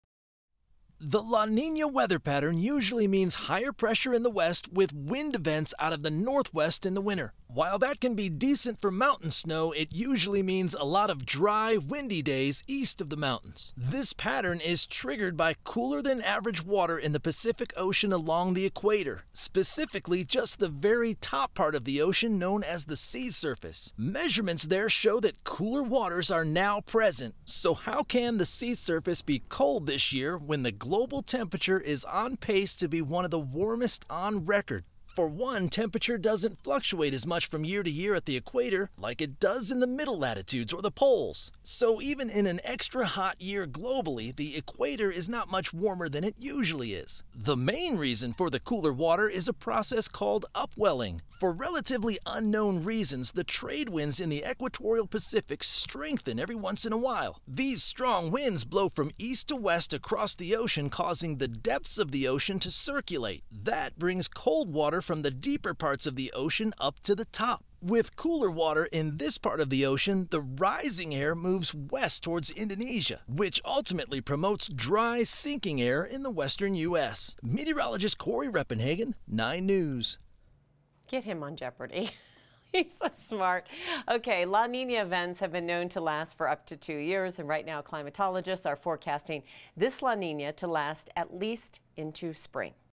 Why is ocean water cold if Earth could have one of warmest years on record?
Meteorologist Cory Reppenhagen has the answer.

9NEWS (KUSA) is located in Denver, Colorado.